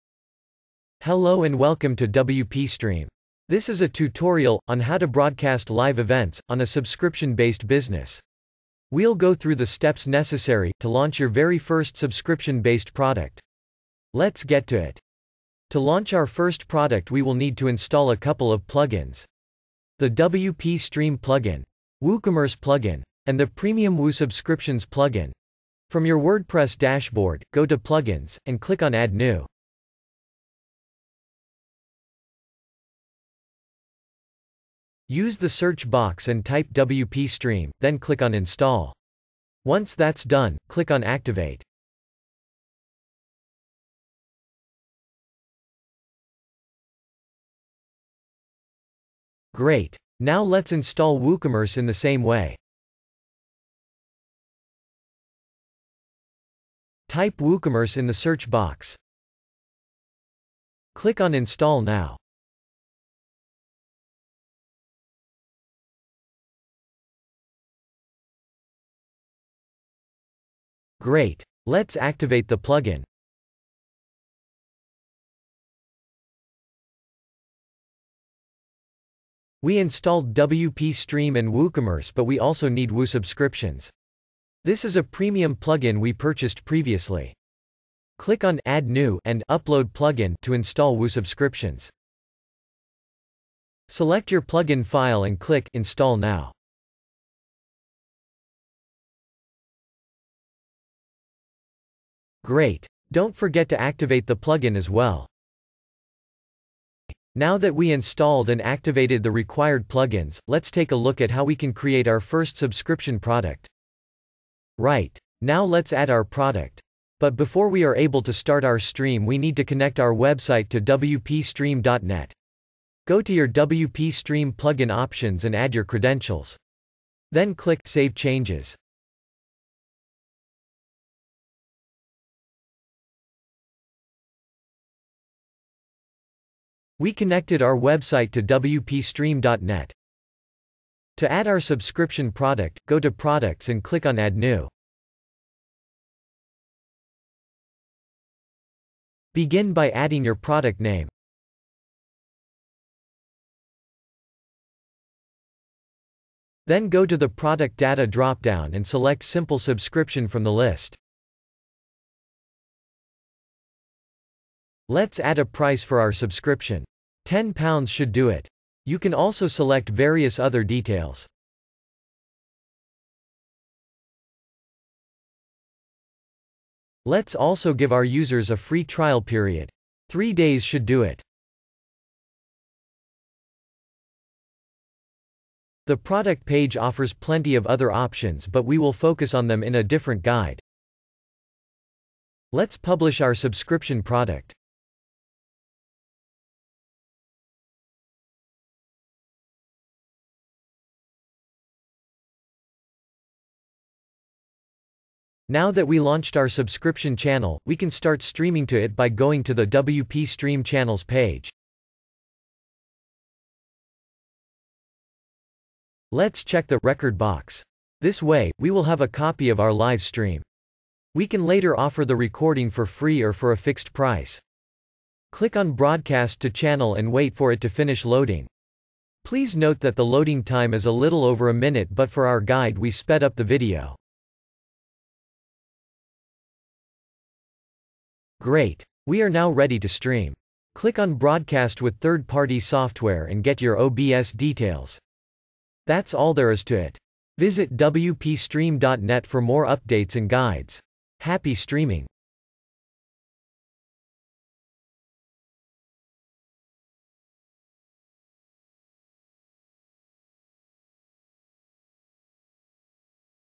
How To Broadcast Live Events On A SUBSCRIPTION BASED BUSINESS
This is a tutorial on how to broadcast live events on a subscription based business with the WpStream platform.